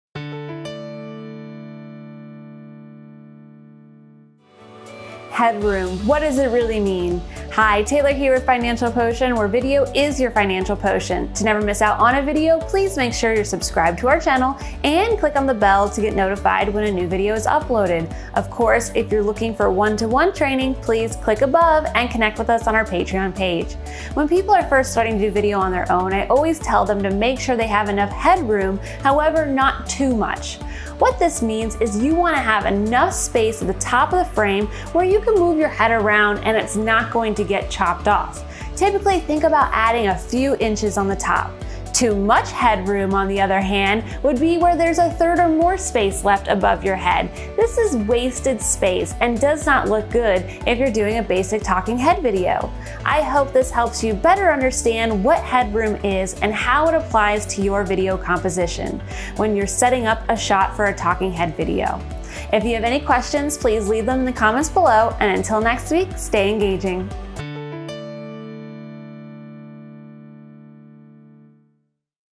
How To Set Up Your Composition for Head- Room? What does Head Room Mean?
Learn how to set up your video composition for head-room and get a quick definition of what head room means. Want videos done for you? Connect with the Financial Potion team

When people are first starting to do video on their own I always tell them to make sure they have enough head room but not too much.

What this means is you want to have enough space at the top of the video frame where you could move your head around and it’s not getting chopped off. Typically think about adding a few inches on the top. Too much head room on the other hand would be where there is a third or more of space above your head. This is wasted space and does not look good if you’re doing a basic talking head video.

Of course sometimes people want to get the extreme close ups where you do chop a bit of the head off, however typically you don't want to do that.

I hope this better helps you understand what is head room, and how it applies to your video composition when you’re setting up a shot. If you have any questions please leave them in the comments below and until next week, stay engaging.